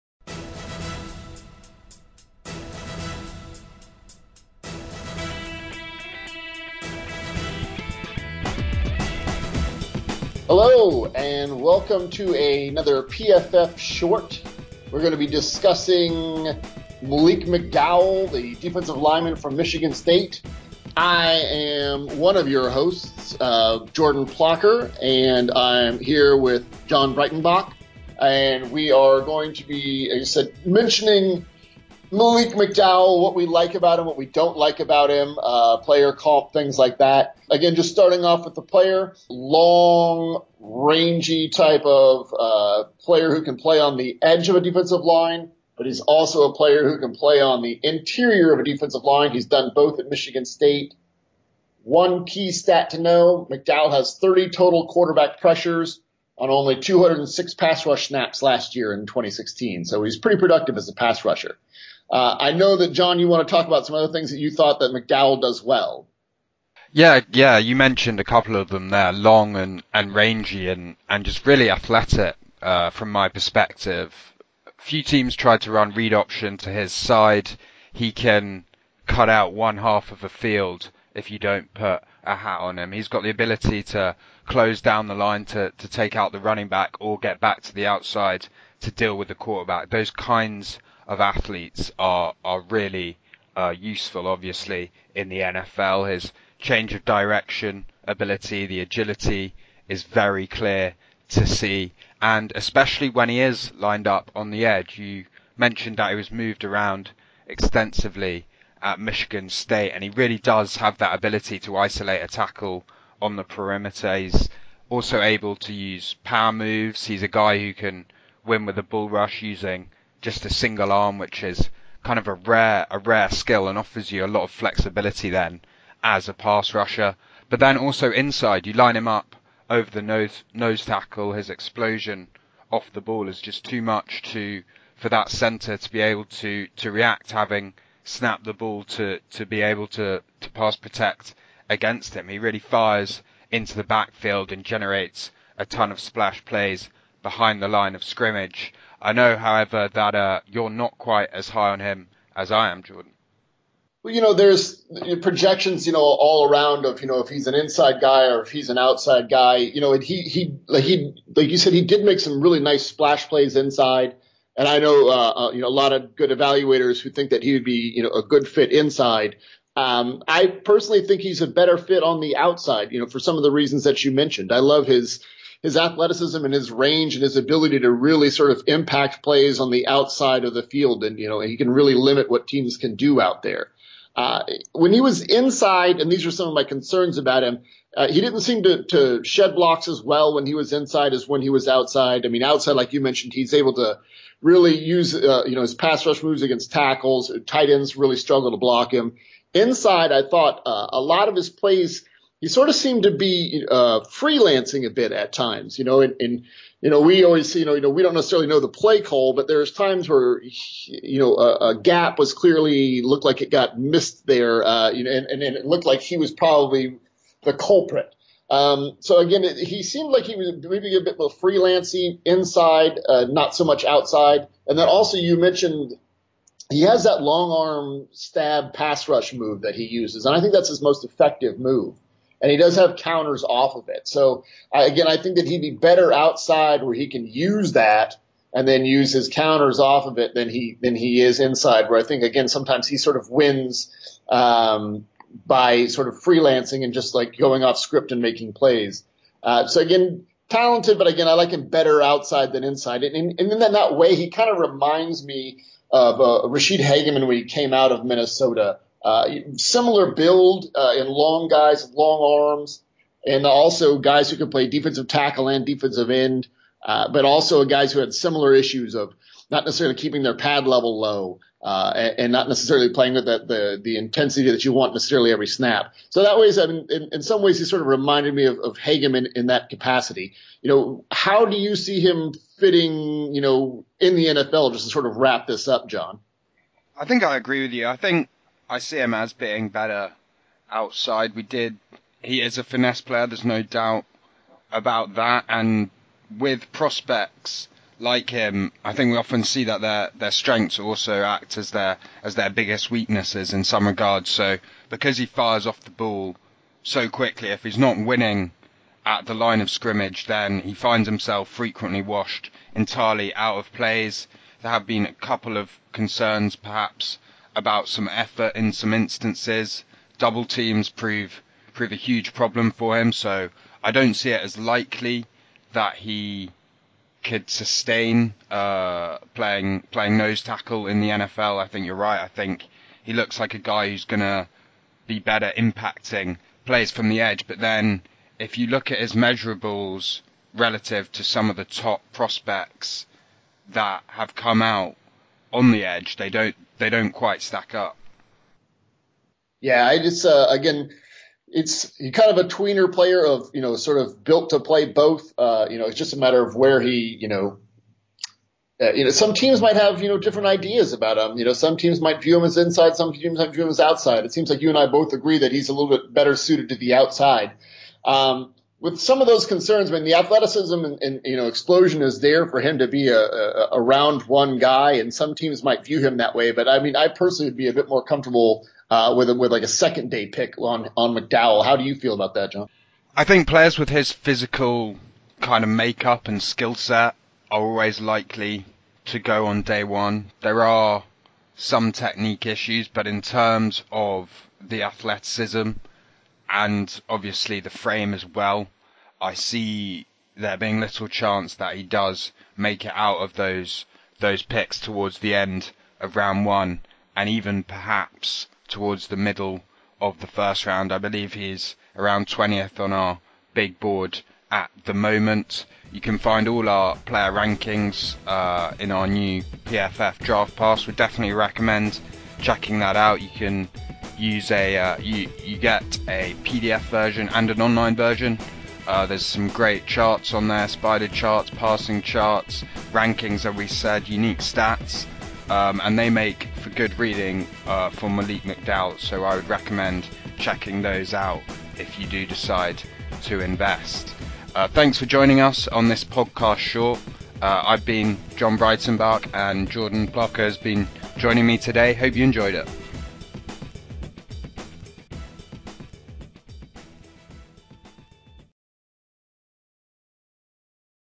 Draft Short: Malik McDowell - DL - Michigan State - 21st Overall Prospect
Jordan and John talk about Malik McDowel's versatility&nbsp;as well as&nbsp;what type&nbsp;of player they see him as in the NFL.